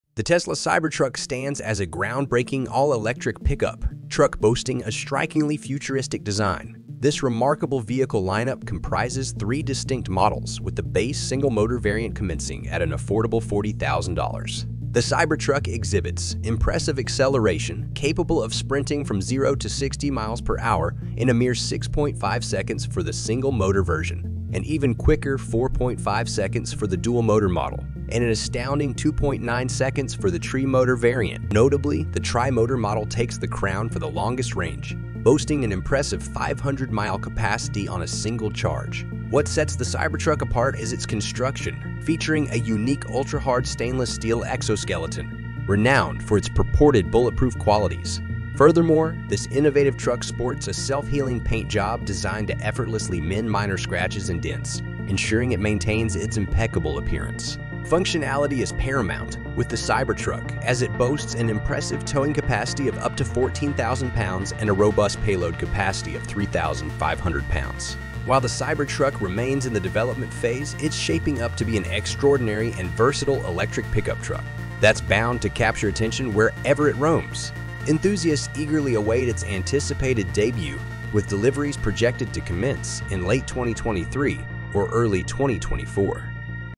2024 Tesla Cybertruck: The Future of Electric Pickup Trucks
"Get ahead of the curve with the 2024 Tesla Cybertruck! 🚀 Join us for an in-depth look at the future of electric vehicles, cutting-edge tech, and jaw-dropping design.